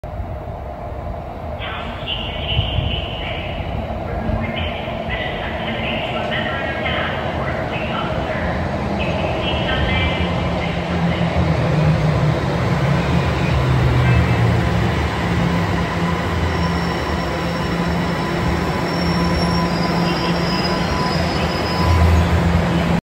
TTC Toronto Rocket 6051 Arriving at Downsview Park Station (no headlights working)
Note: Headlights are not working